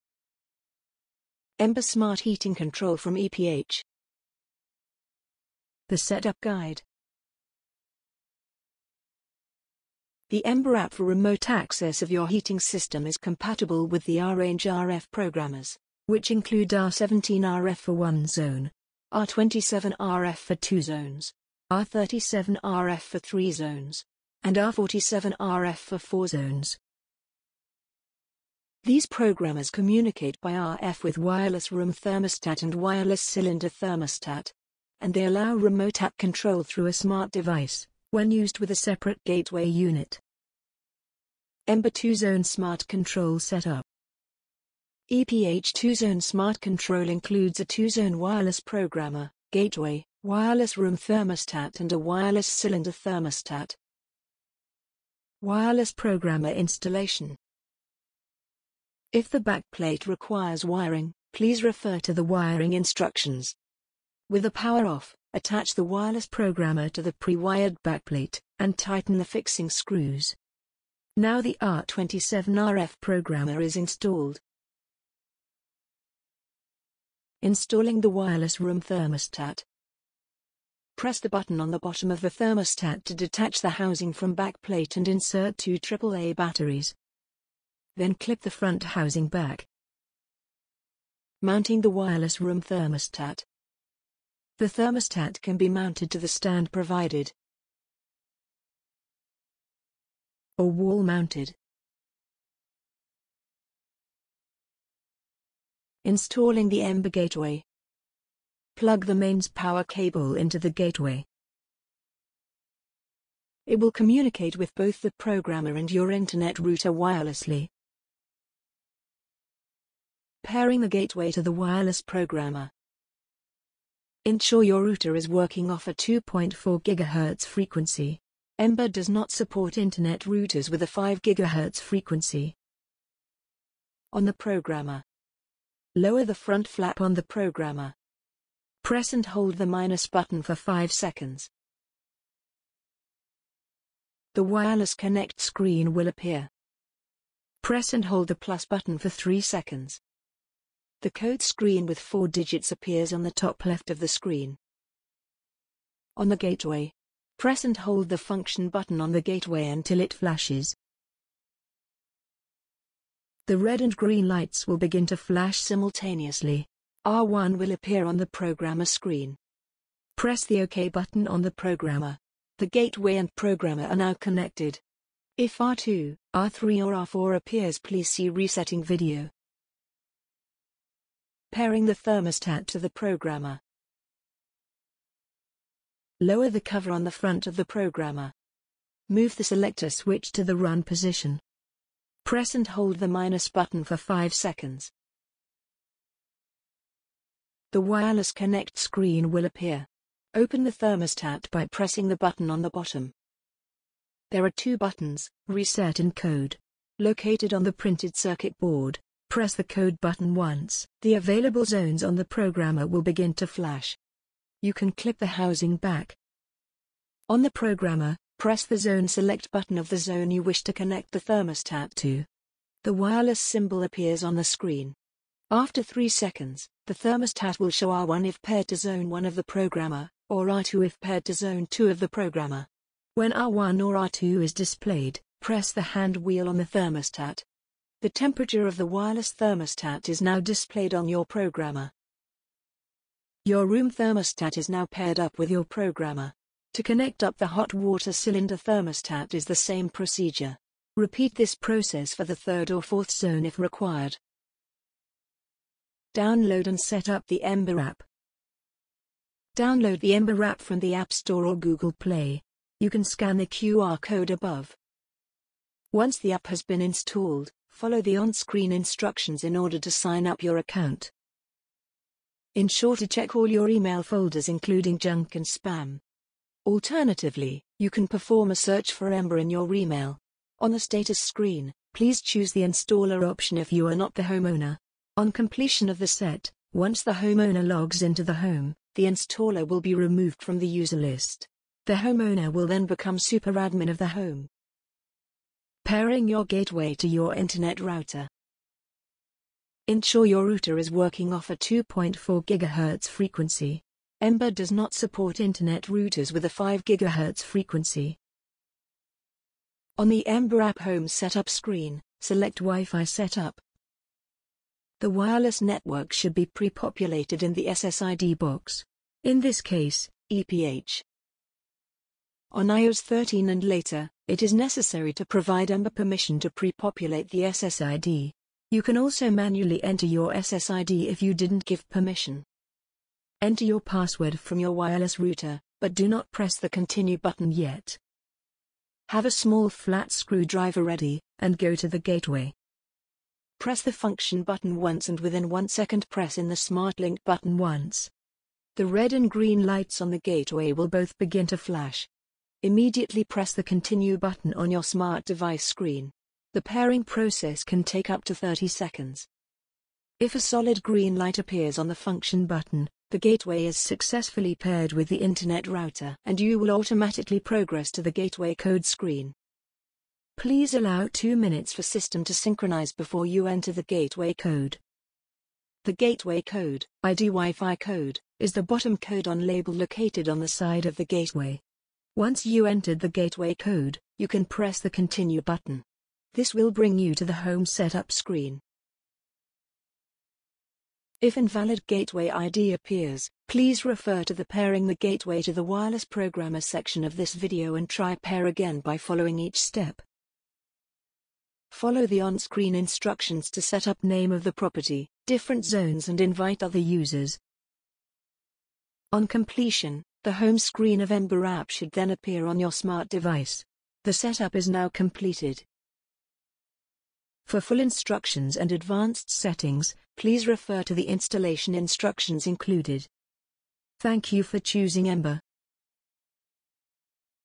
How to Set up EMBER PS Smart Heating Control App
The EMBER PS Setup Guide shows installers how to install the EMBER PS Smart Heating Control system with a step by step guide. This guide covered the following area:
1) Components of EMBER smart heating control and how they work together
2) Installation of Programmer, Thermostats and the EMBER Gateway. (start at 00:57 of this video)
3) Pairing the Gateway to the Wireless Programmer (start at 02:00 of this video)
4) Pairing the Thermostat to the Programmer (start at 03:00 of this video)
5) Download and Set up the EMBER App (start at 04:15)
6) Pairing your gateway to your internet router (start at 04:55)